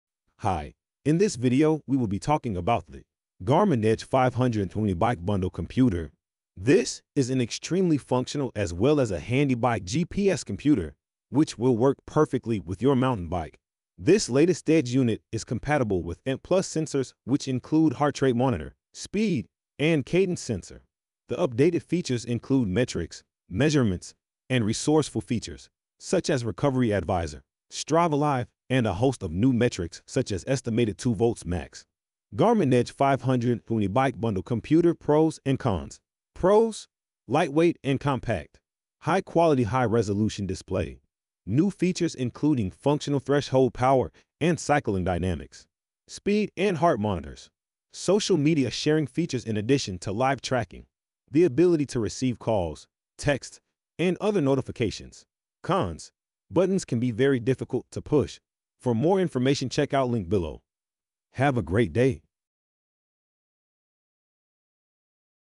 Garmin Edge 520 Bike Bundle Computer
This is an extremely functional as well as a handy bike GPS computer, which will work perfectly with your mountain bike. This latest Edge unit is compatible with ANT+ sensors which include heart rate monitor, speed, and cadence sensor. The updated features include metrics, measurements, and resourceful features, such as Recovery Advisor, Strava Live and a host of new metrics such as estimated VO2 Max.